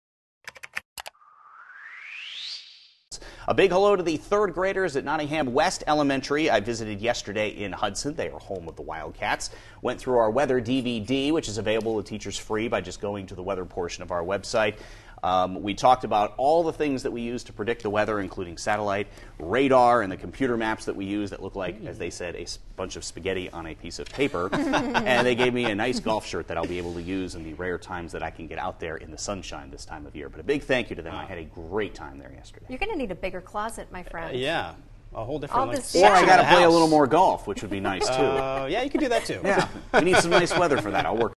Nottingham West Elementary
Meteorologist Kevin Skarupa visited Nottingham West Elementary.